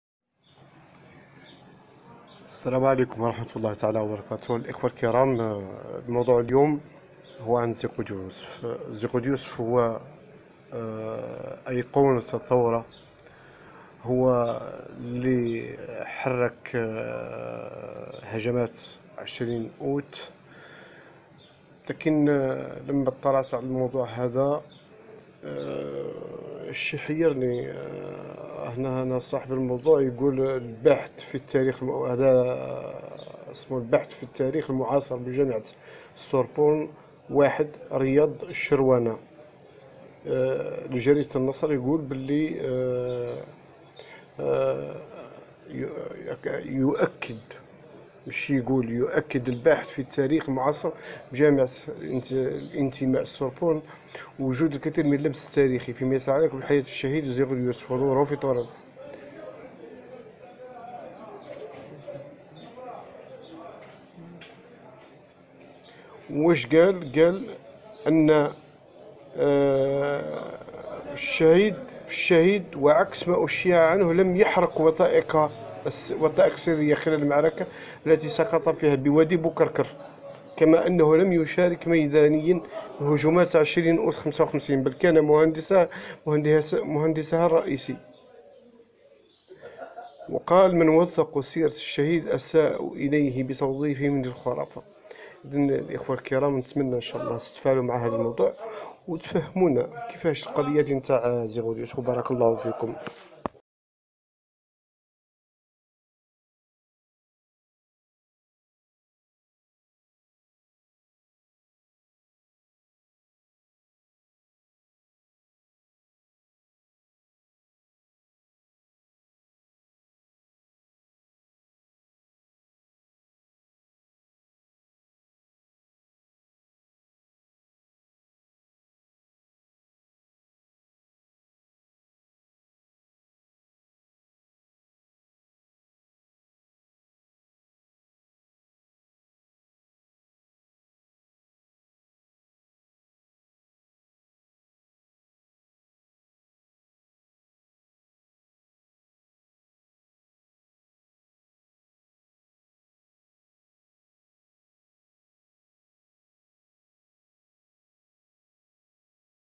زيغود يوسف هندس لهجمات 20 اوت ولكنه لم يشارك فيها ...أشياء أخرى مثيرة.
كل يوم  يظهر لنا كلام  من هنا  وهناك ..كلام نشاز فهذا المؤرخ يدرس في جامعة  السوربون  أي في فرنسا  عدوة زيغود  يوسف بكل المقاييس.